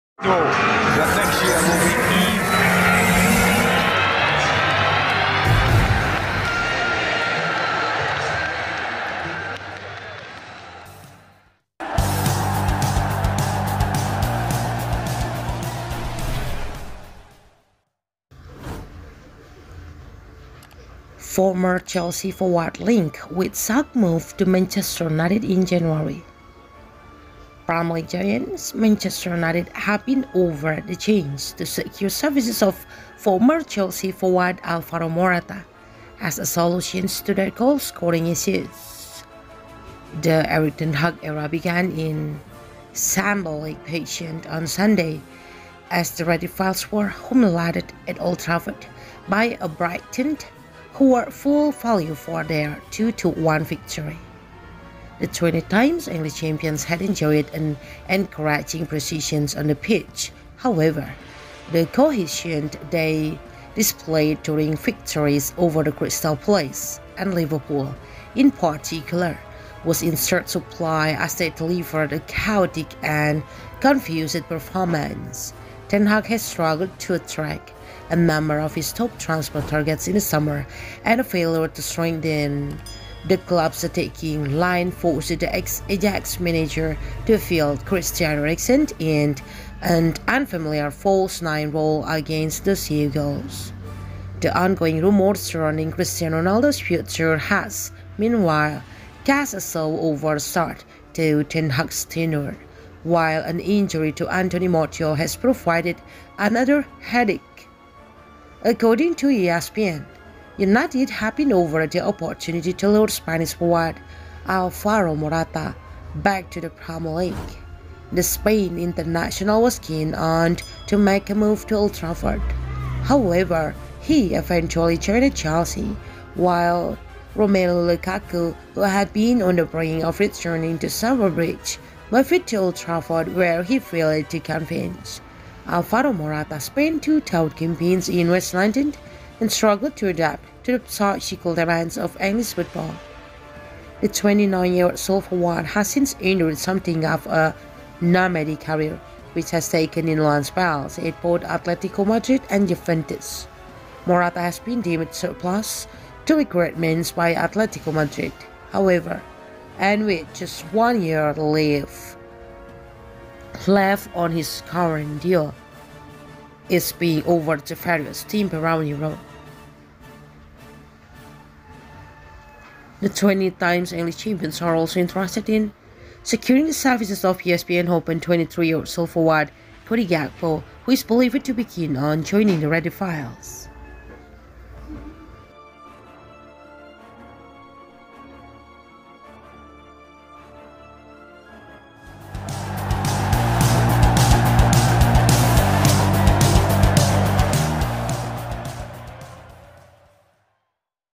'BIG SHOCK MOVE': Former Chelsea forward agreed with shock move to Man Utd in January
Former Chelsea forward linked with shock move to Man Utd in January

Premier League giants, Manchester United have been offered the chance to secure services of former Chelsea forward Alvaro Morata as a solution to their goalscoring issues.

The Erik ten Hag era began in shambolic fashion on Sunday as the Red Devils were humiliated at Old Trafford by a Brighton who were full value for their 2-1 victory.

The 20-times English champions had enjoyed an encouraging pre-season on the pitch, however, the cohesion they displayed during victories over the Crystal Palace and Liverpool, in particular, was in short supply as they delivered a chaotic and confused performance.

Ten Hag has struggled to attract a number of his top transfer targets in the summer and a failure to strengthen the club’s attacking line forced the ex-Ajax manager to field Christian Eriksen in an unfamiliar false nine role against the Seagulls.

The ongoing rumours surrounding Cristiano Ronaldo’s future has, meanwhile, cast a show over the start to Ten Hag’s tenure, while an injury to Anthony Martial has provided another headache.

According to ESPN, United have been offered the opportunity to lure Spanish forward Alavaro Morata back to the Premier League.

The Spain international was keen on to make a move to Old Trafford, however, he eventually joined Chelsea, while Romelu Lukaku, who had been on the brink of returning to Stamford Bridge, moved to Old Trafford where he failed to convince.

Alvaro Morata spent two tough campaigns in west London and struggled to adapt to the physical demands of English football.

The 29-year-old forward has since endured something of a nomadic career which has taken in loan spells at both Atletico Madrid and Juventus.

Morata has been deemed suprlus to requirements by Atleico Madrid, however, and with just one year left on his current deal is being offered to various team around Europe.

The 20-times English champions are also interested in securing the services of PSV Eindhoven’s 23-year-old forward Cody Gakpo who is believed to be keen on joining the Red Devils.